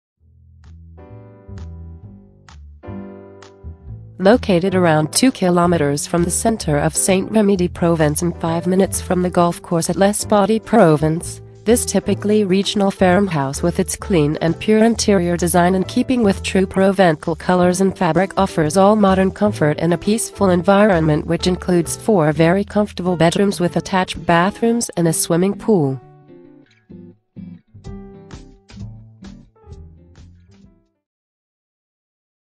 St Rémy de Provence - Le Mas de Manon
Located around 2 kms from the centre of St Rémy-de-Provençe and 5 mins from the golf course at Les Baux-de-Provençe, this typically regional farmhouse with its clean and pure interior design in keeping with true Provençal colours and fabric offers all modern comfort in a peaceful environment which includes four very comfortable bedrooms with attached bathrooms and a swimming pool.